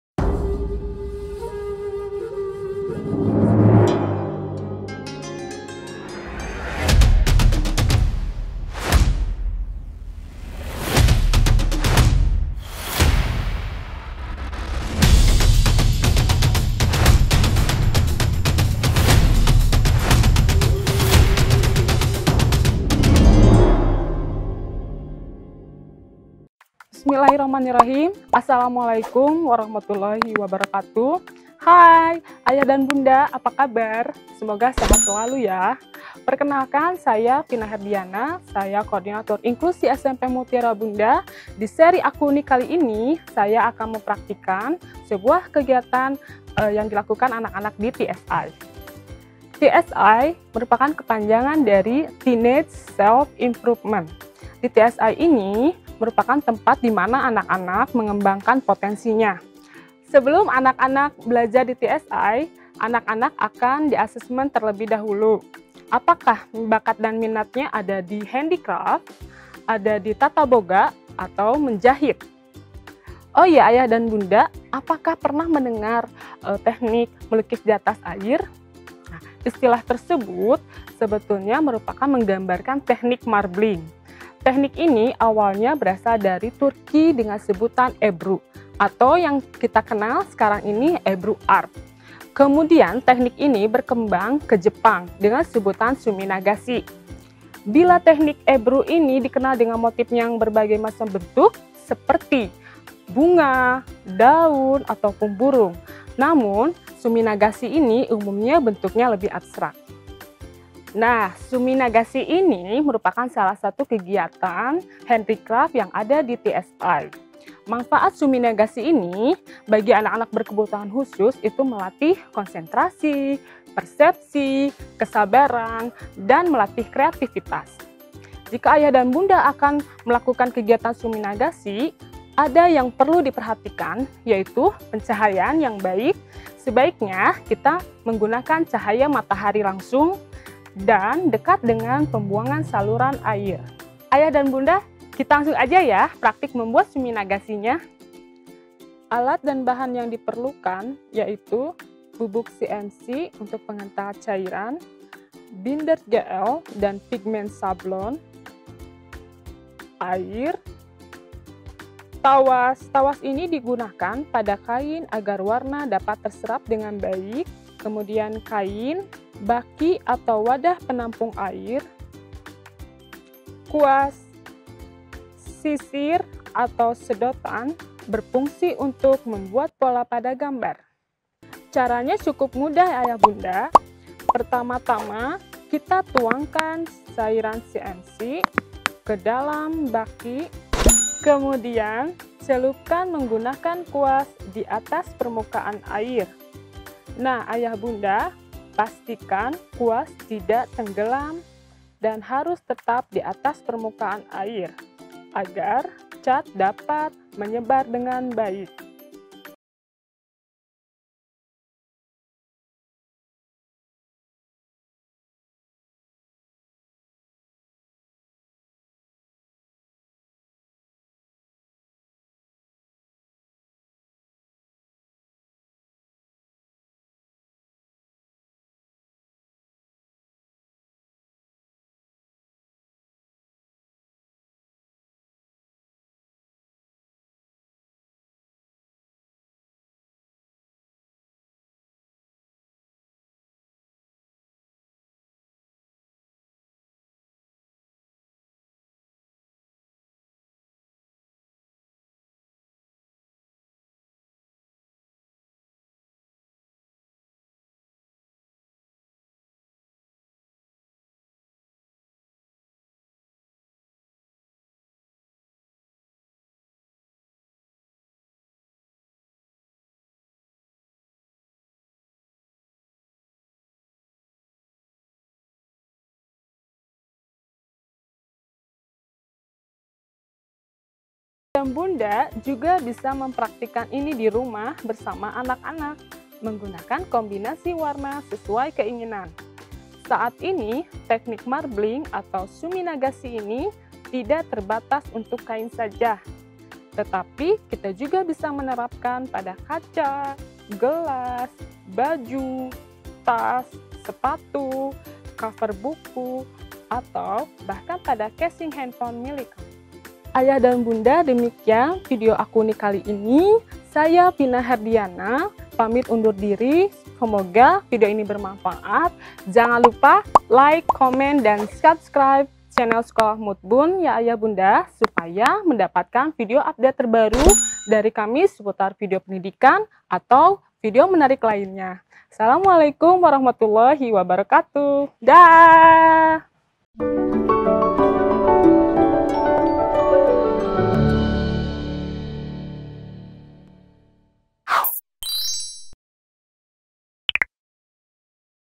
Suminagashi Art Tutorial Pada Kain | Aku Unik ! | Sekolah Mutiara Bunda
Assalamu'alaikum Mutbunders!
Tetap Semangat di tahun 2021!

Edisi Aku Unik! kali ini kita akan belajar mewarnai kain dengan teknik Suminagashi bersama Bu Fina Herdiana (Koordinator Inklusi SMP Mutiara Bunda).

Mari kita simak, Semoga bermanfaat ❤

Follow us for all the updates and Information!